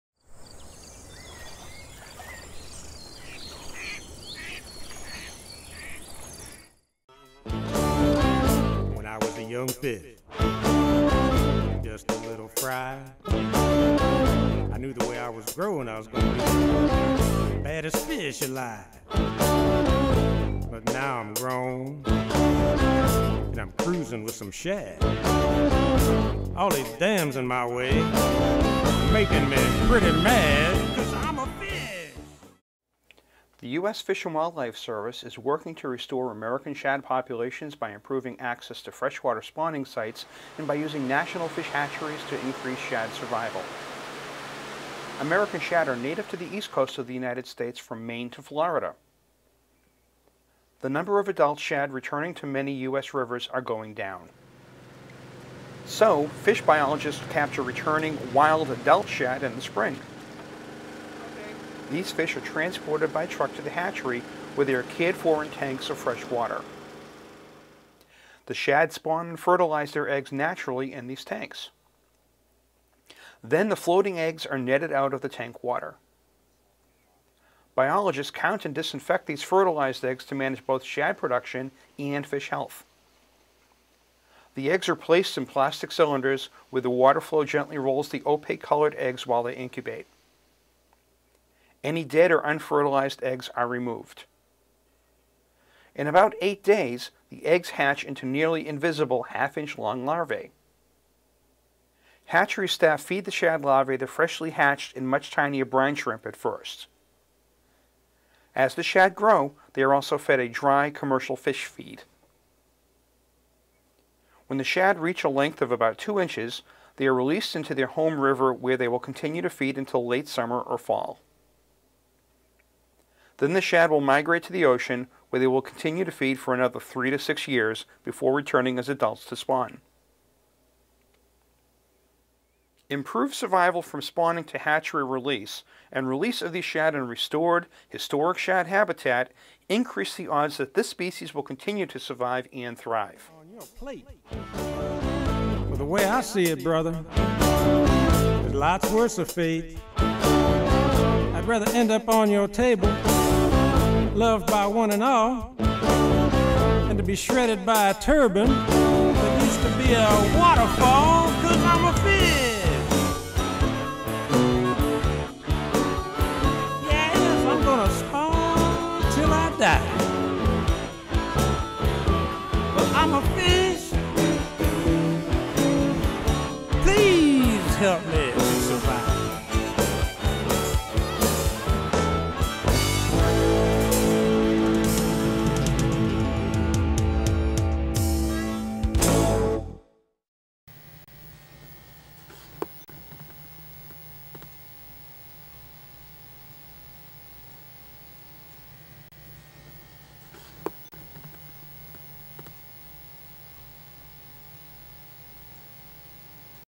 American Shad Restoration-Using National Fish Hatcheries to Conserve Shad
A short video describing the role that our National Fish Hatcheries play in American Shad Restoration.